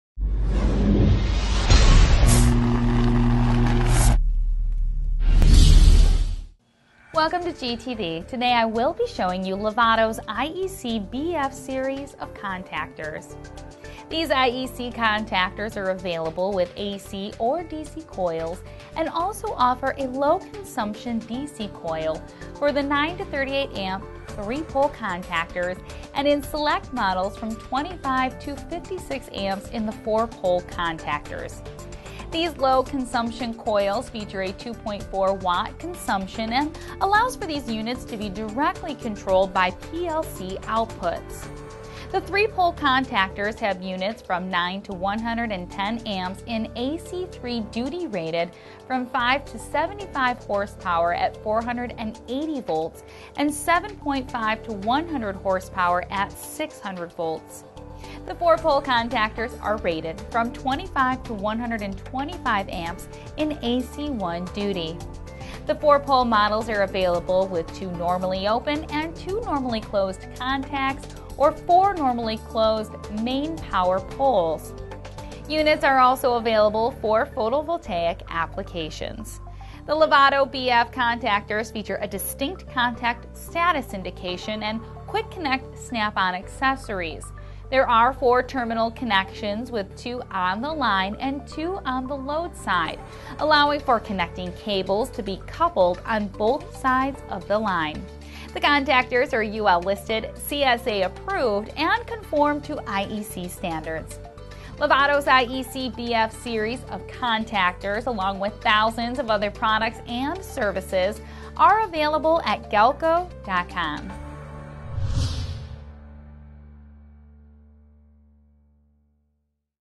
Lovato BF Series IEC Contactors
Lovato BF Series IEC Contactors presented by Katie Nyberg for Galco TV.

BF Series Features:
-    Low Consumption Version for Control Relays
-    Highly Conductive Auxiliary Contacts
-    Types for Photovoltaic Applications
-    Distinct Contact Status Indication
-    AC or DC Auxiliary Supply
-    Mechanical Interlock